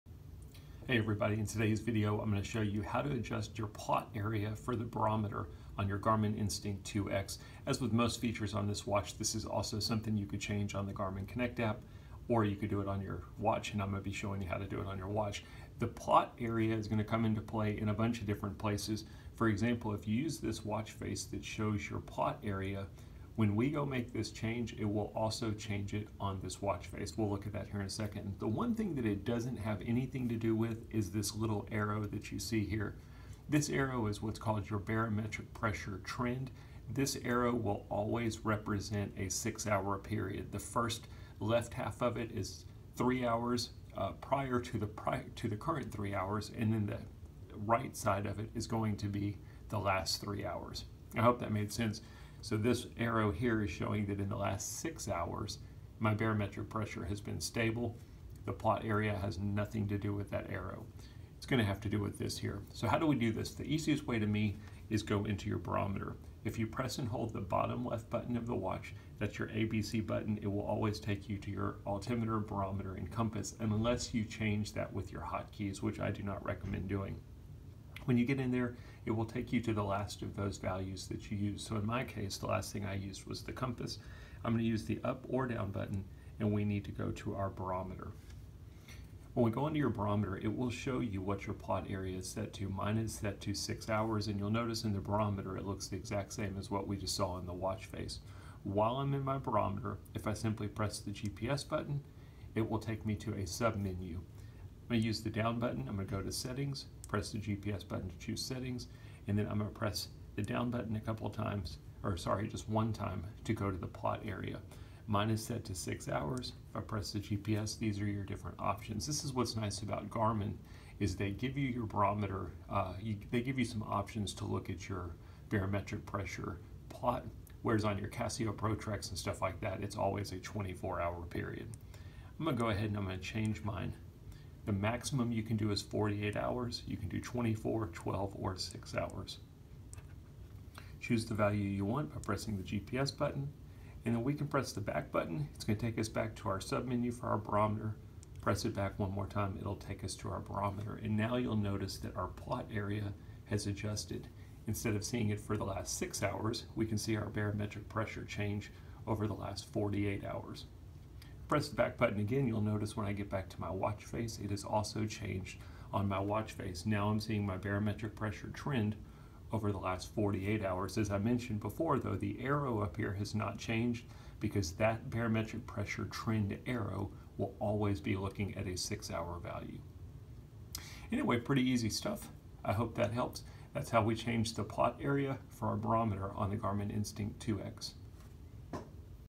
Garmin Instinct 2X | Adjust Barometer Plot Area
Garmin Instinct 2X Solar ~ How to adjust the barometer plot area for controlling the duration of the barometric pressure levels you can see.

Hope the video helped, thumbs up always appreciated!